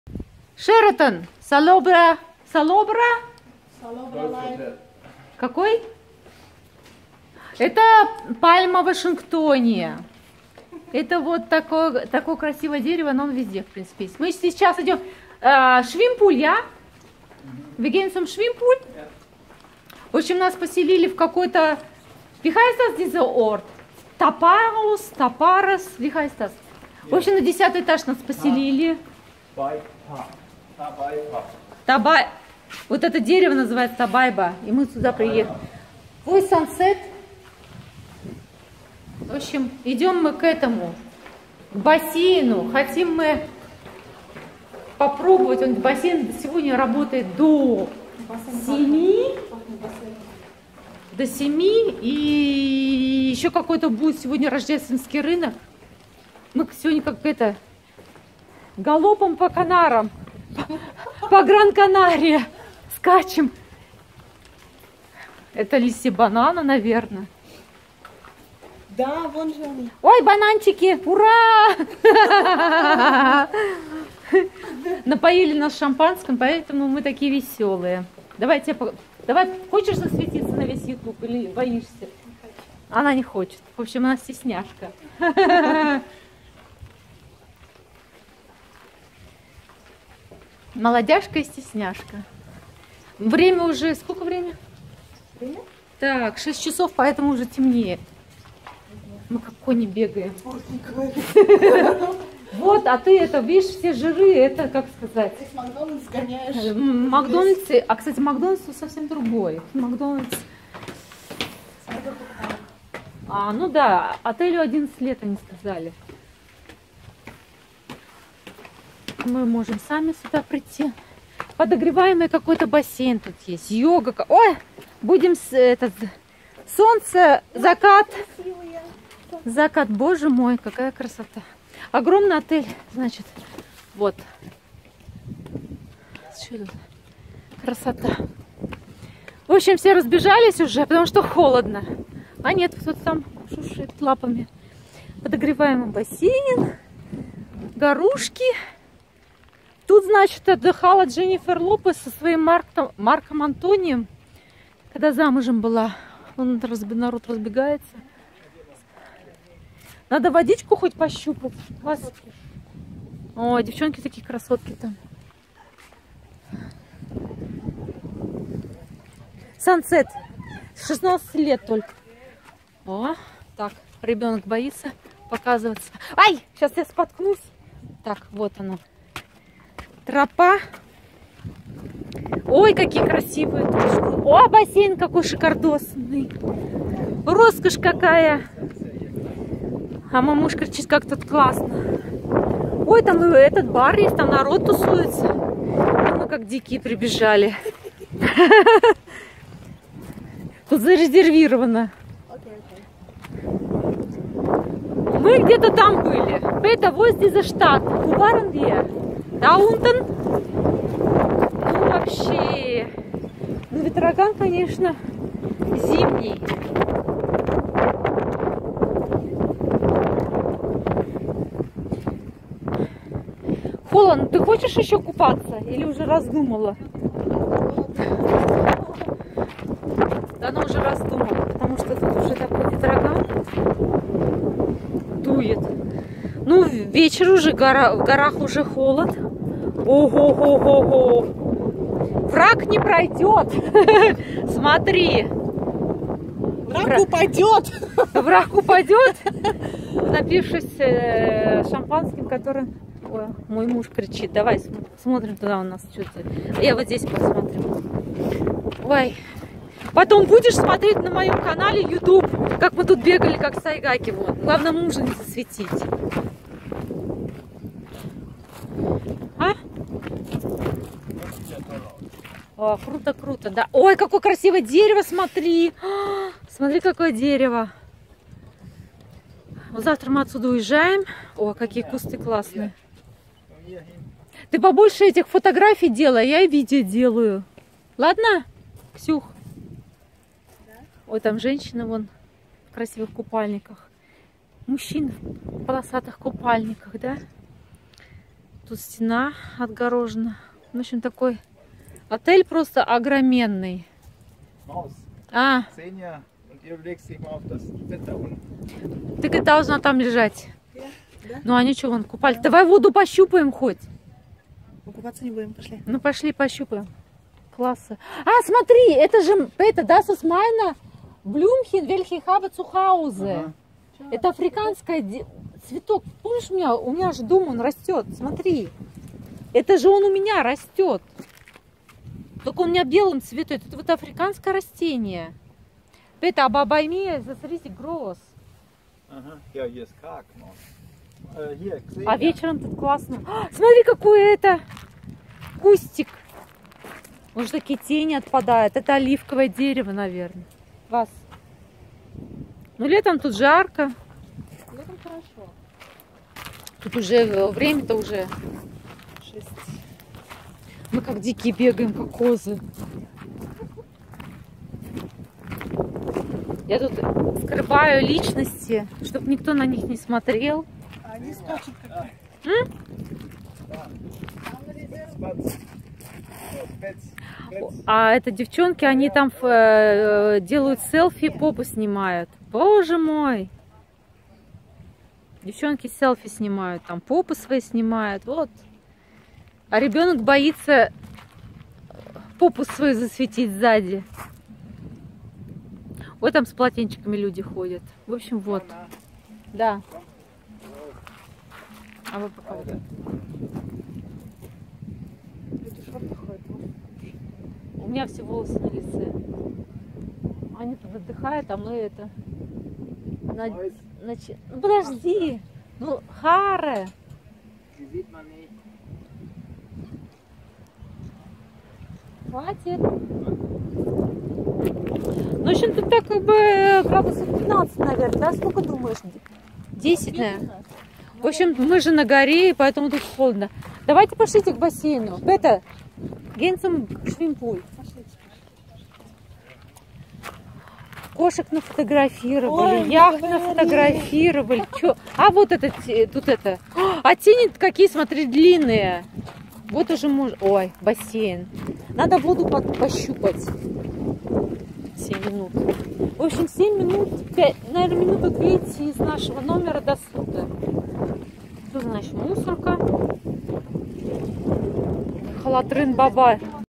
Sheraton Gran Canaria Salobre Golf Resort 5*, Гран-Канария.Закат. Декабрь.
Флора острова Гран-Канария.
Гран-Канария, Канарские острова, Sheraton Gran Canaria Salobre Golf Resort.